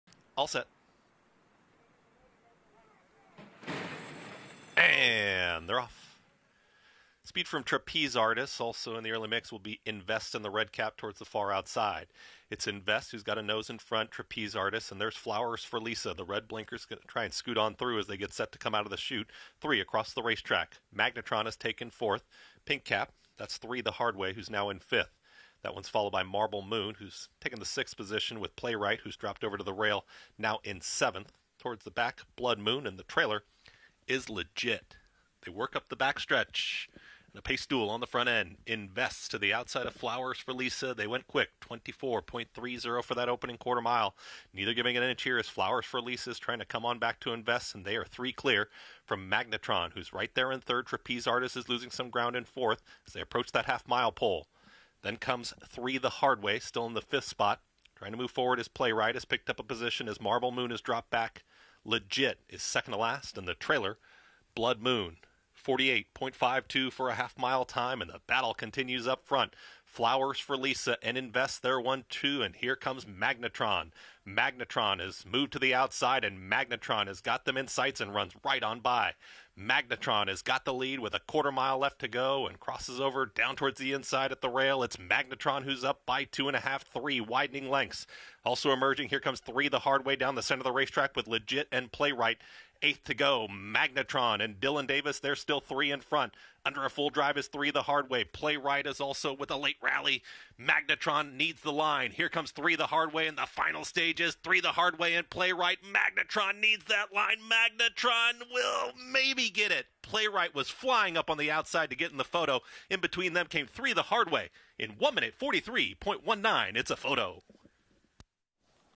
Flowers for Lisa at Aqueduct 1/16/22
Flowers for Lisa finished 8th in the 7th Race at Aqueduct on 1/16/22